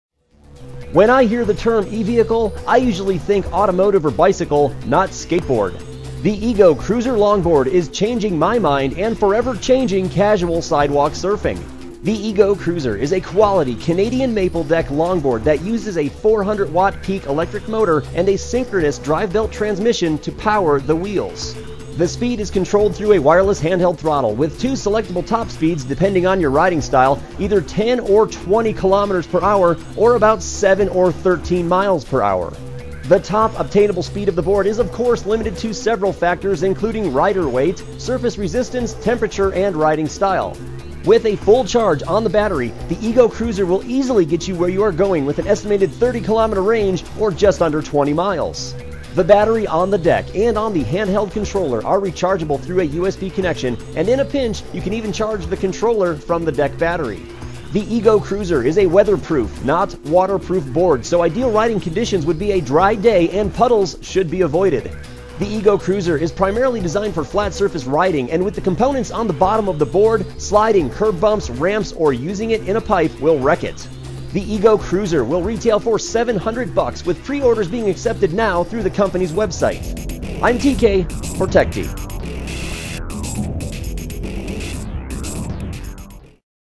Motorized Coasting on an E-Go Cruiser Electric Longboard
The E-Go Cruiser is a quality Canadian maple deck longboard that uses a 400-watt peak electric motor and a synchronous drive-belt transmission to power the wheels. The speed is controlled through a wireless hand-held throttle with two selectable top speeds depending on your style, either 10 or 20 kilometers per hour, or about 7 or 13 miles per hour. The top obtainable speed of the board is of course limited to several factors including rider weight, surface resistance, temperature and riding style. With a full charge on the battery, the E-Go Cruiser will easily get you where you are going with an estimated 30-kilometer range, or just under 20 miles. The battery on the deck and on the hand-held controller are rechargeable through a USB connection and in a pinch, you can even charge the controller from the deck battery. Check them out